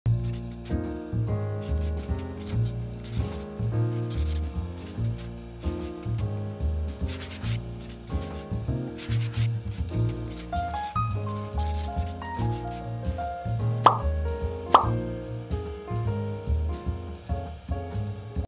Writing Math for Exams Tom
Remember from the previous video when i said im going to take a break, yea never that was a lie, anyways besides me lying, i decided to use Krita for doing solutions for Math lol